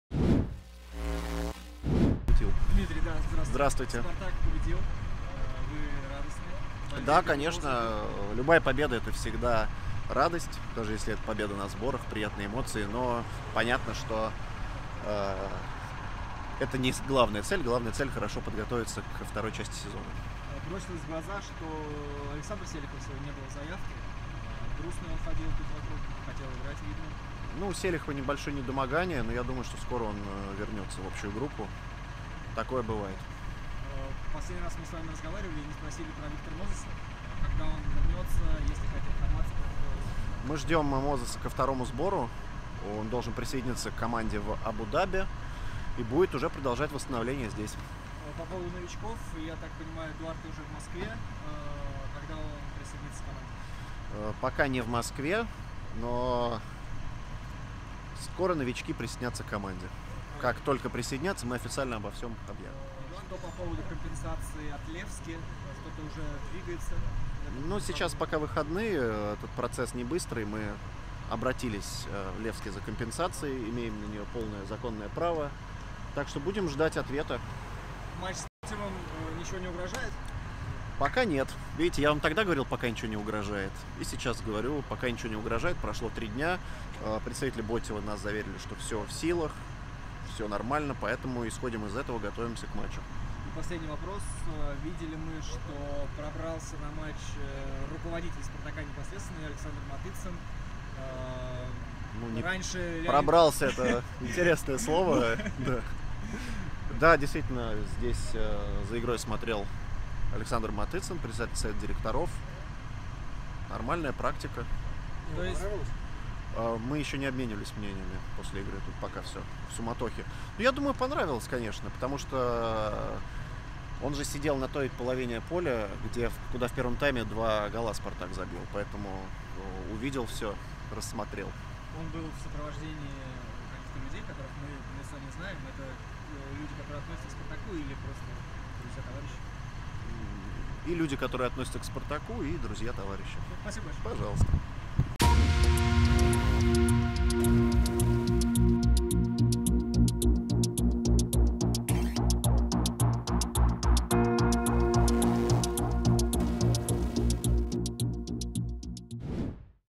СПАРТАК: Первая игра на сборах / Визит Матыцына / Когда вернется Мозес?
«Спартак» провел первую игру в году, обыграв на сборах узбекский клуб АГМК со счетом 3:1. После матча руководитель пресс-службы красно-белых Дмитрий Зеленов ответил на насущные вопросы — о возвращении травмированного Виктора Мозеса, визите главы совета директоров клуба Александра Матыцына и приезде новичков.